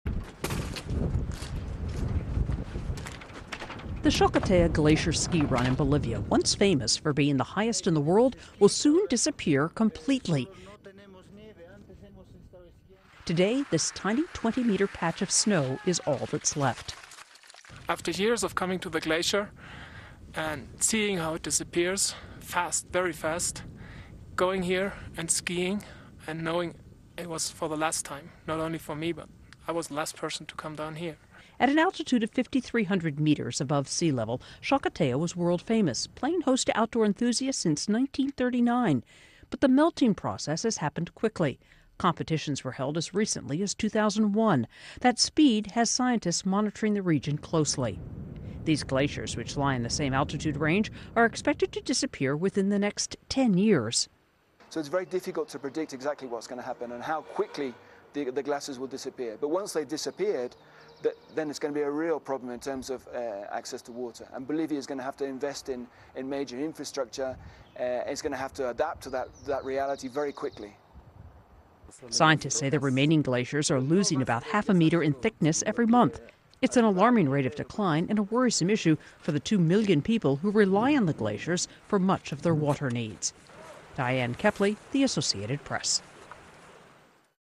Disappearing Glaciers: Bolivia's Chacaltaya
The Chacaltaya glacier ski run in Bolivia  - once famous for being the highest in the world - is set to disappear completely within the next few months. (Dec. 7)